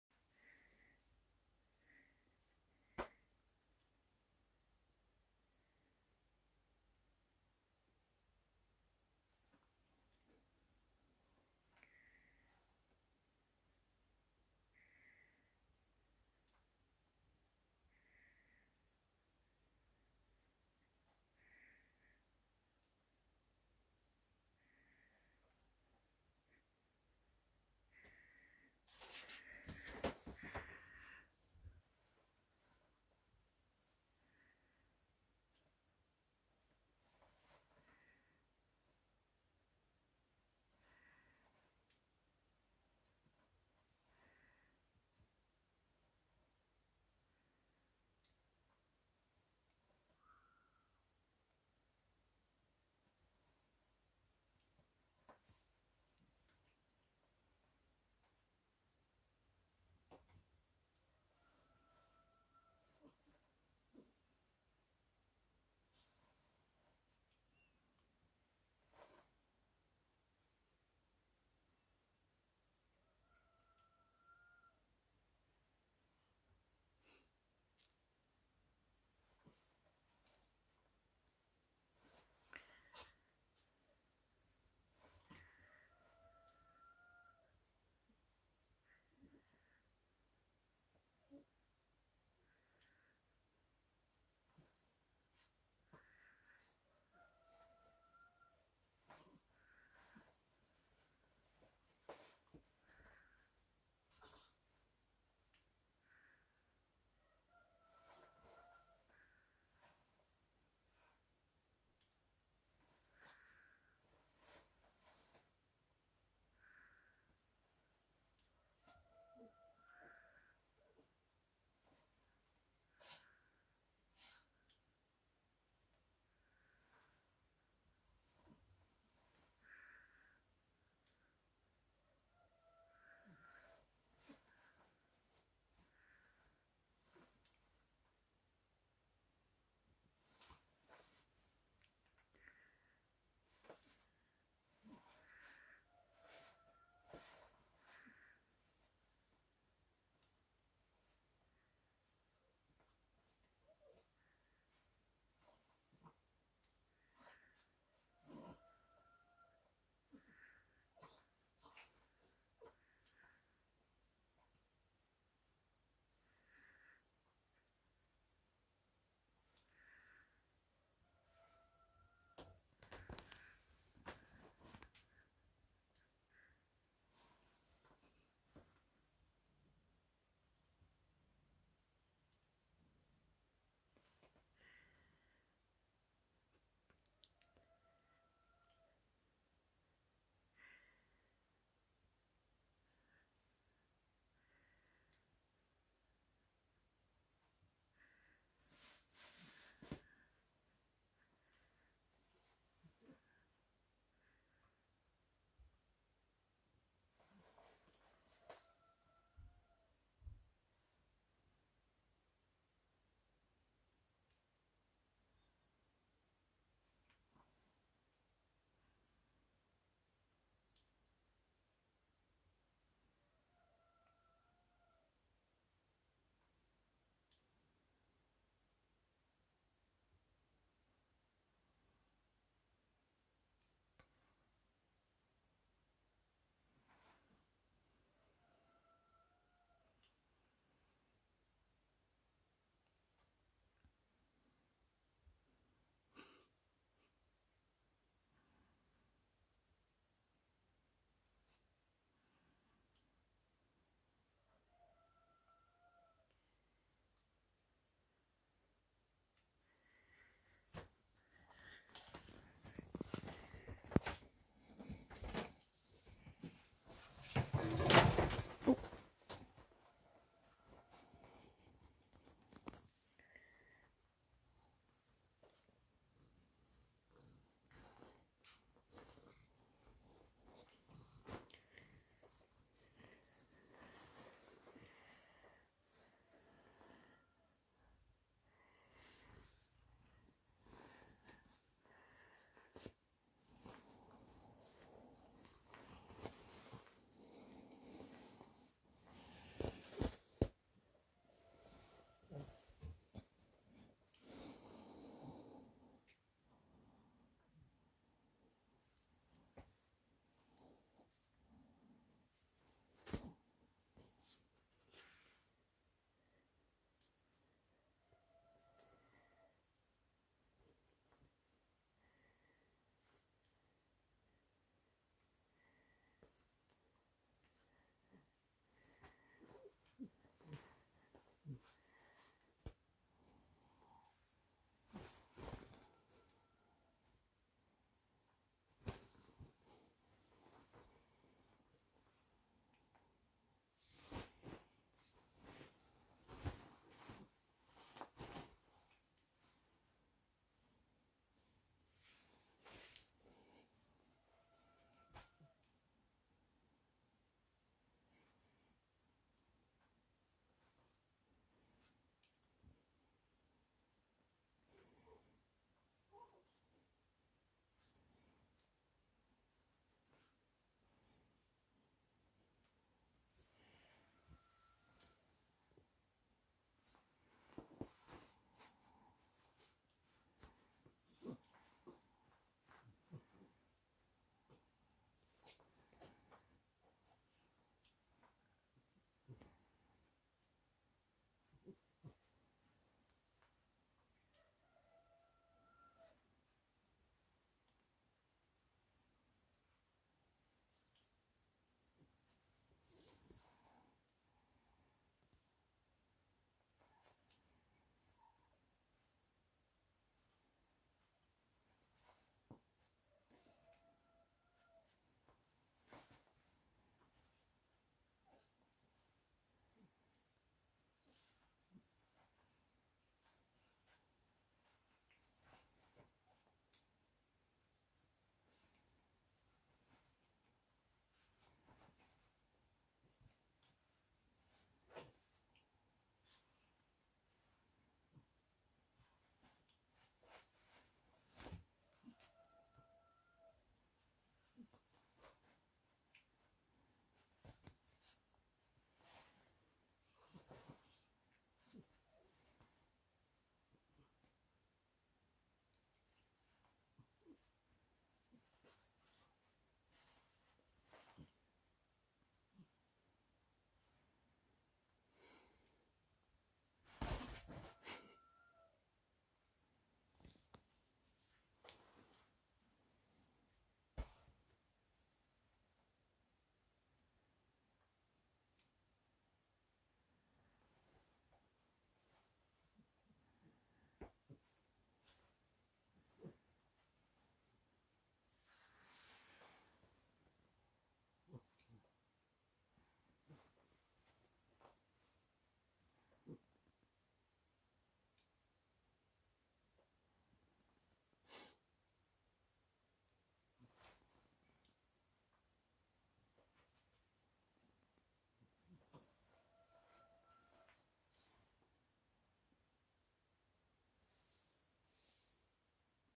New Cat's first catnip toy
I finally found my stash of catnip and stuffed some of the toys I knit months ago. This is probably New Cat's first encounter with catnip.
New Cat showed up at my place this summer, meowing for help, so I gave him food and slowly won his trust. Now he sleeps on my bed. I suppose I'll have to actually name him soon.